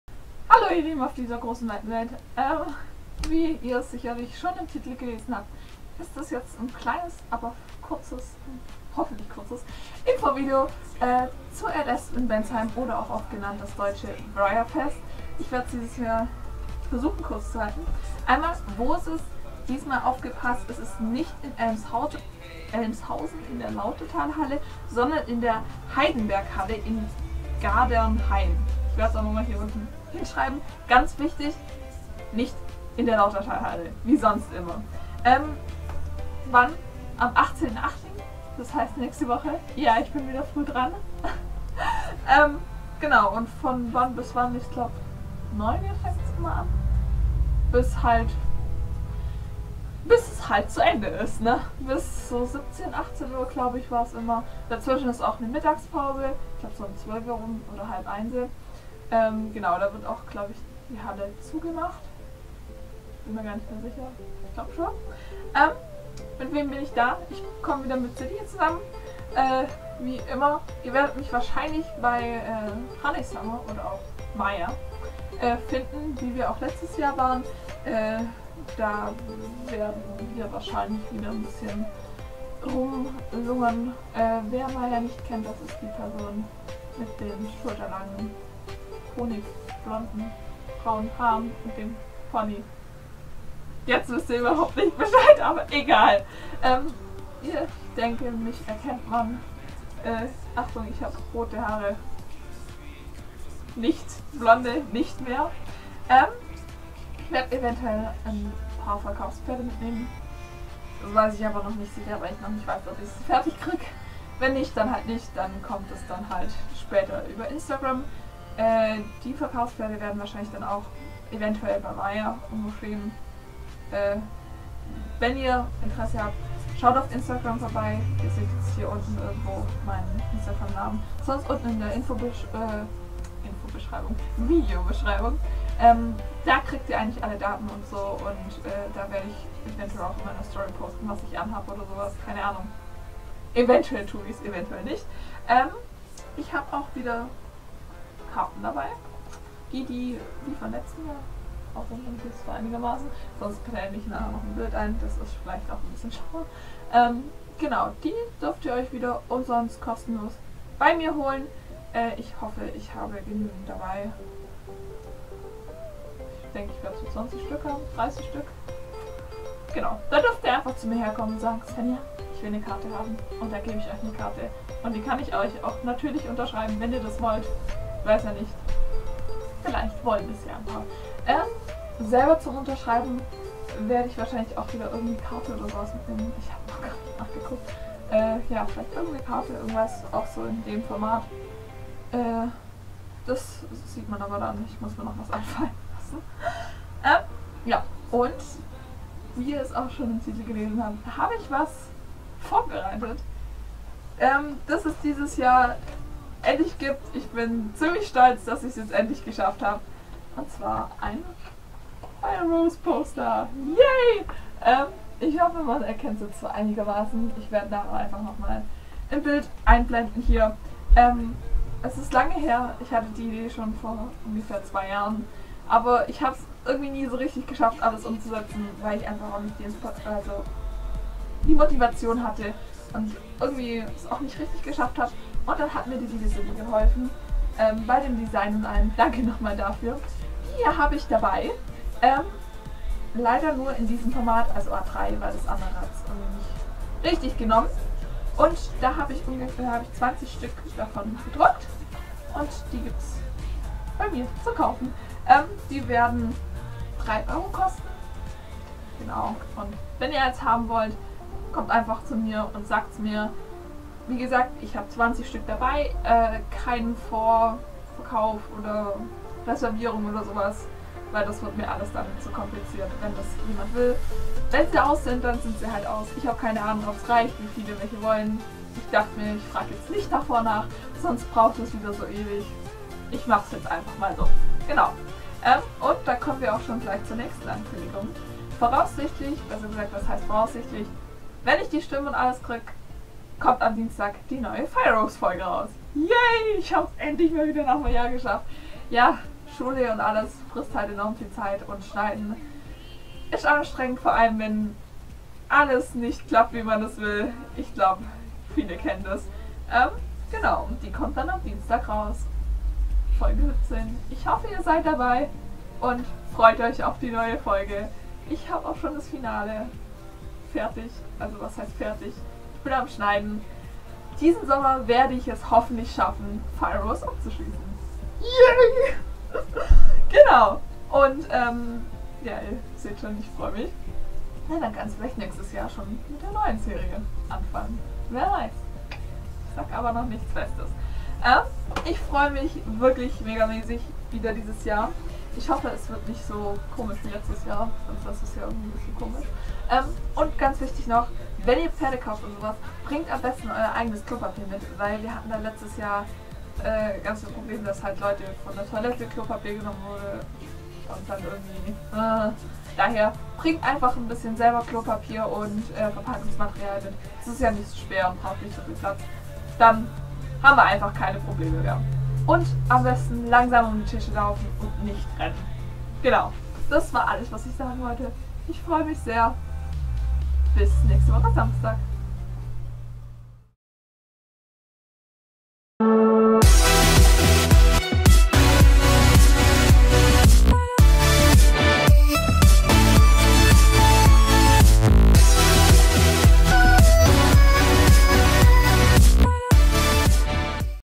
Bensheim 2018~ ANKÜNDIGUNG/ÜBERRASHUNG!!
YESSSSS Bensheim 2018!!
Ich freu mich riesig! Mal sehen was ich so alles finde...
Vielleicht finde ich auch den ein oder anderen Body, der später zu einem Verkaufspferd wird... :D
Firerose Poster
- nur in A3
- 3 Euro pro Stück
- keine reservierung/vorverkauf!